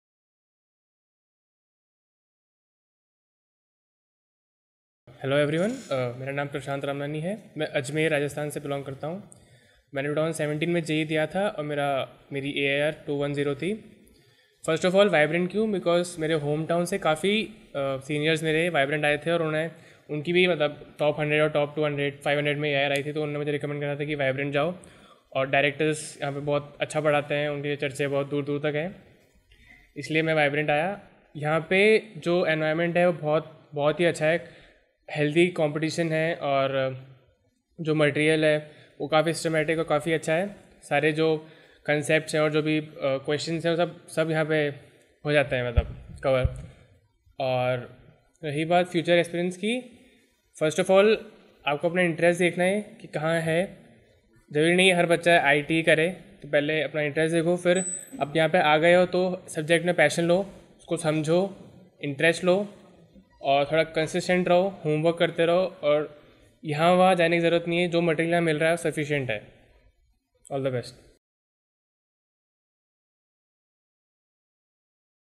आज में वाइब्रेंट की वजह से टॉप रैंक में अपनी जगह बना पाया हूँ
मेरे होम टाउन सीनियर्स ने मुझे वाइब्रेंट एकडेमी ज्वाइन करने के लिए कहा और आज में भी उन्ही की तरह वाइब्रेंट की वजह से टॉप रैंक में अपनी जगह बना पाया हूँ और IIT - KHARAGPUR से इंजीनियरिंग कर रहा हूँ I

प्रशांत रामनानी , AIR -210 (JEE -2017)
अजमेर, राजस्थान